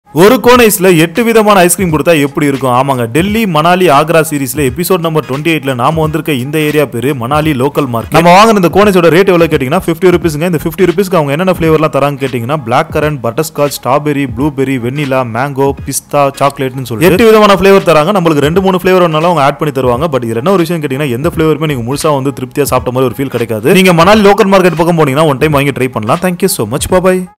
💥 ஒரு Cone🍦la 8 Flavours of ice creams uhhhh ⁉️ thedsquarevlogs Delhi EP - 28
8 in 1 ice cream shop #
In manali famous mall street you will find many street side vendor selling eight flavours of ice cream in one cone,taste wise you will never satisfied but look wise it’s too good.

Address: Manali local market,Mall Road Manali HP,(near Vajbhai statue )#
Cone ice Price : 50/-Rs